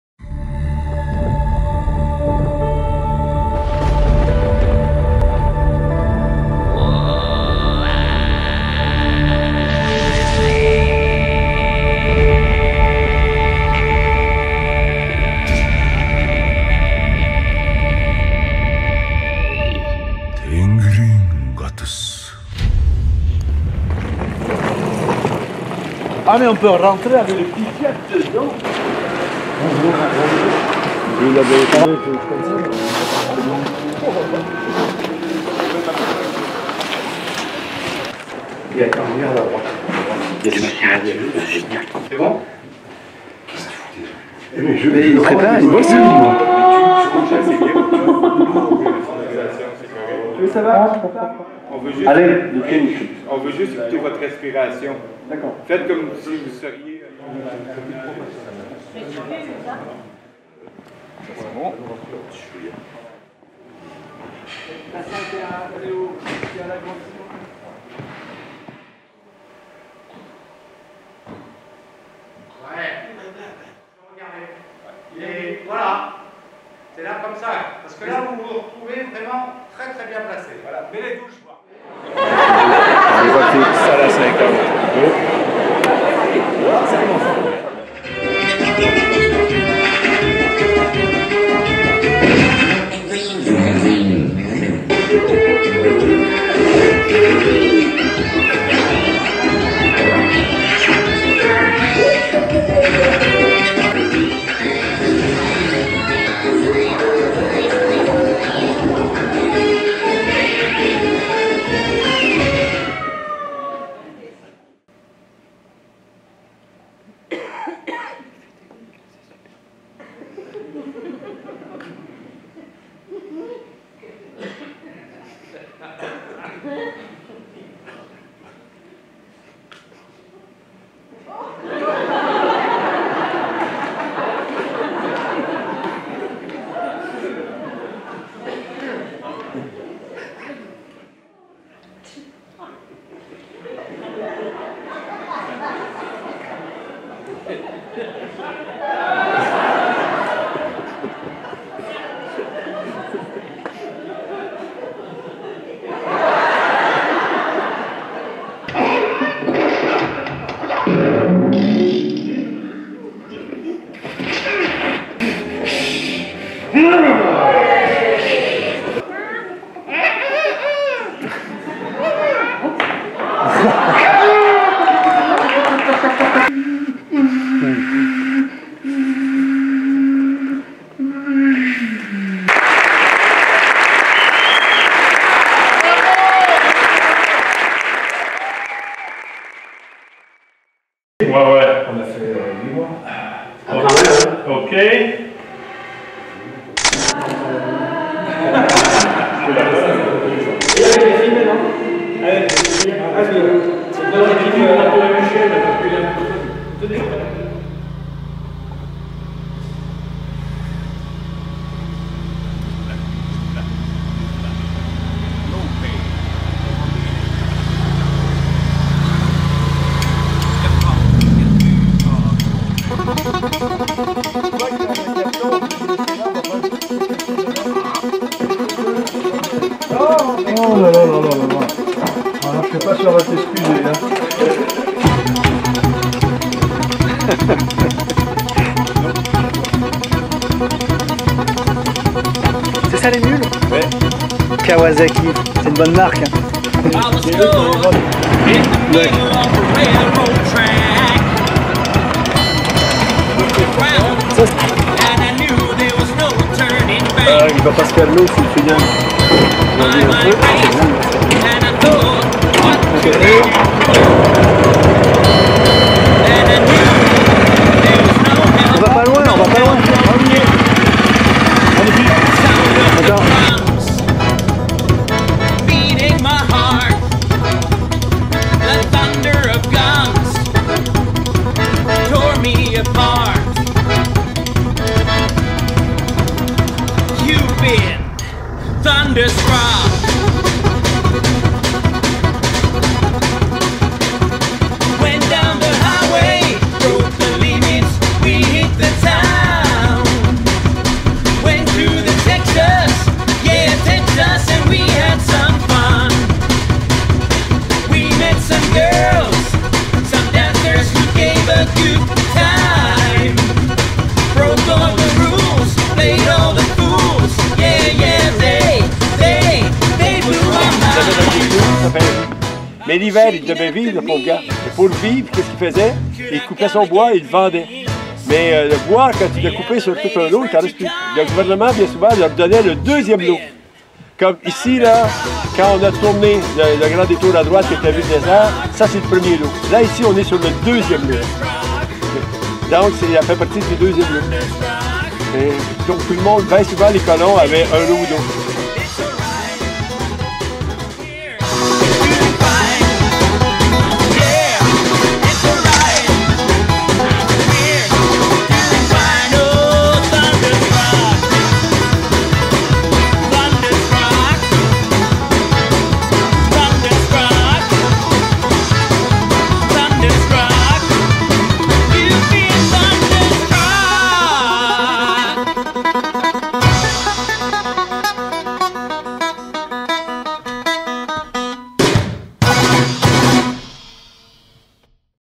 Le jour J de la représentation de KATAÏ 253 à Mont Laurier
Notre journée à Mont Laurier où nous avons présenté KATAÏ 253... du Théâtre, de la joie et de la découverte... Merci Michel !